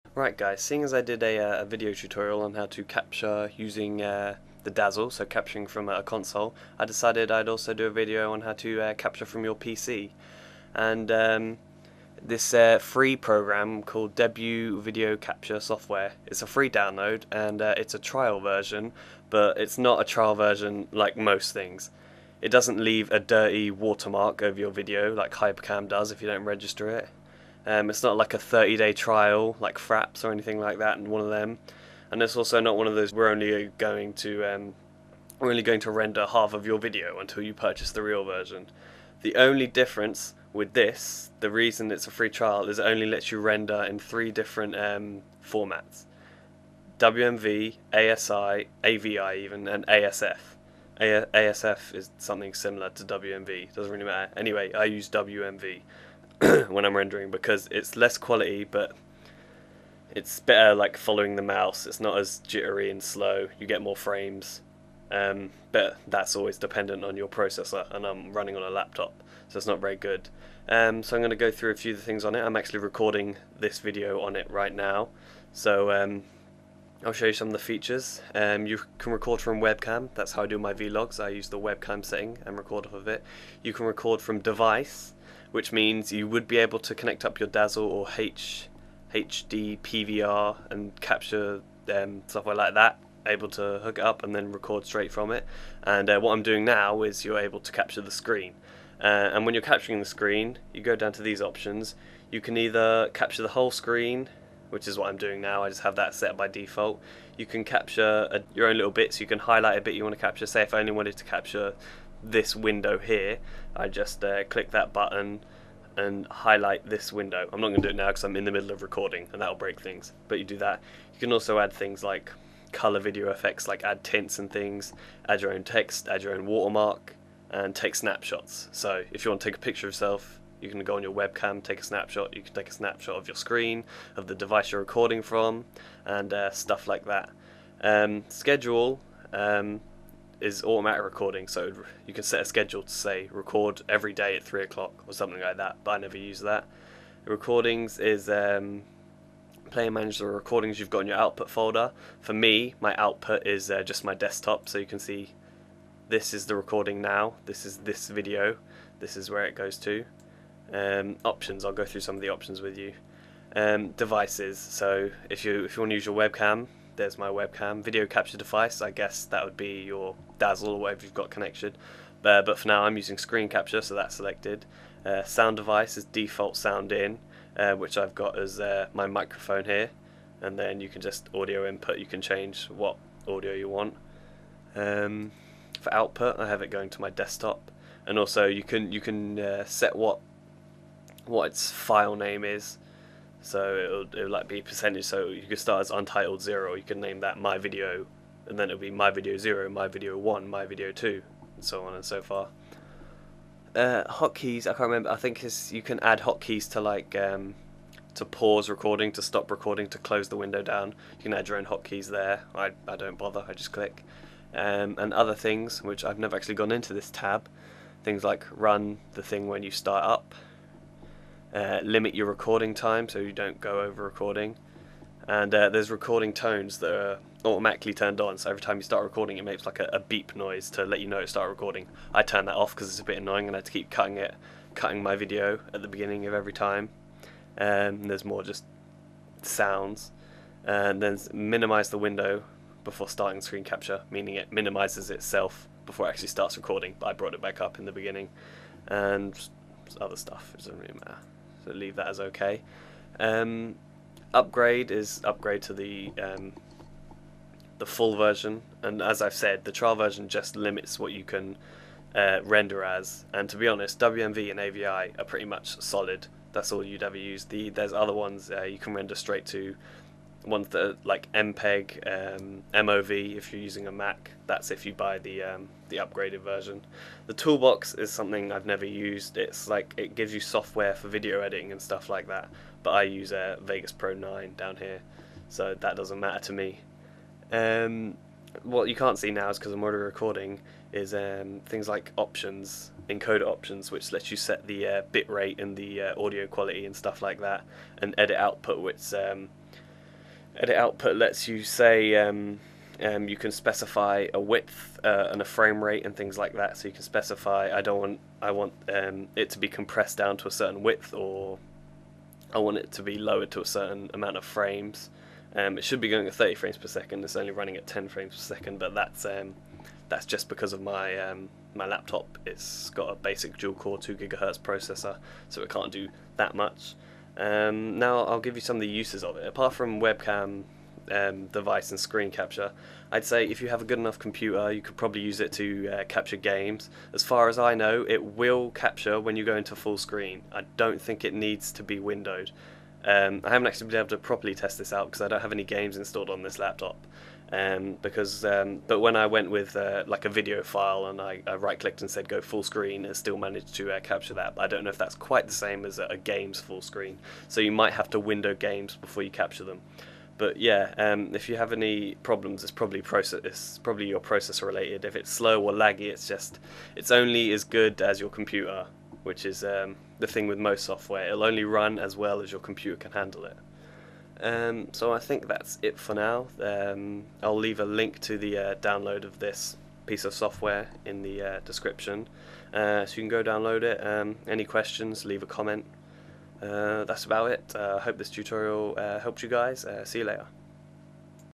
Debut Video Capture Software Tutorial - Free Screen/Webcam/Device Capture and Recording Software
A quick tutorial on how to use this awesome piece of free software.